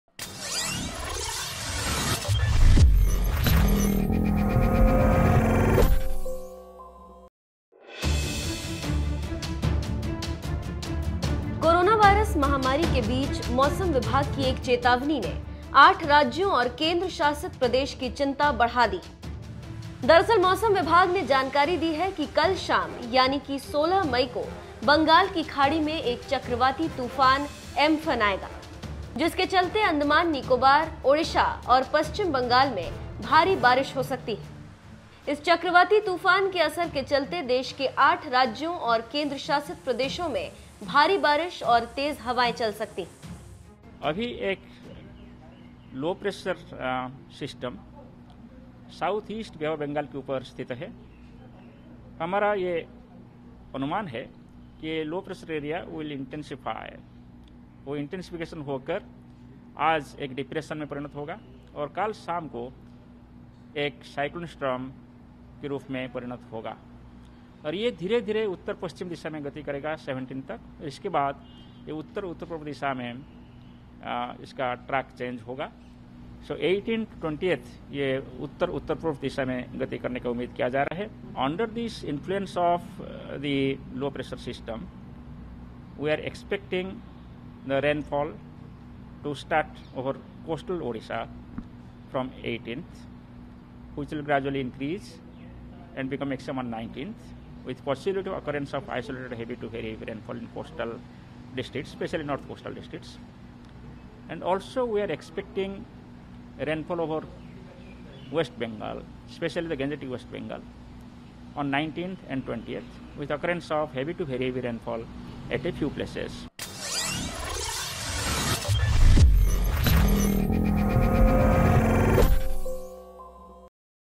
सावधान ! कल आने वाला है भारत के इन राज्यों में तूफान, IMD की चेतावनी
कोरोना वायरस महामारी के बीच मौसम विभाग की एक चेतावनी ने आठ राज्यों और केन्द्र शासित प्रदेश की चिंता बढ़ा दी है… दरअसल मौसम विभाग ने जानकारी दी है कि कल शाम यानी की 16 मई को बंगाल की खाड़ी में एक चक्रवाती तूफान एम्फन आएगा, जिसके चलते अंडमान निकोबार, ओडिशा और पश्चिम बंगाल में भारी बारिश हो सकती है…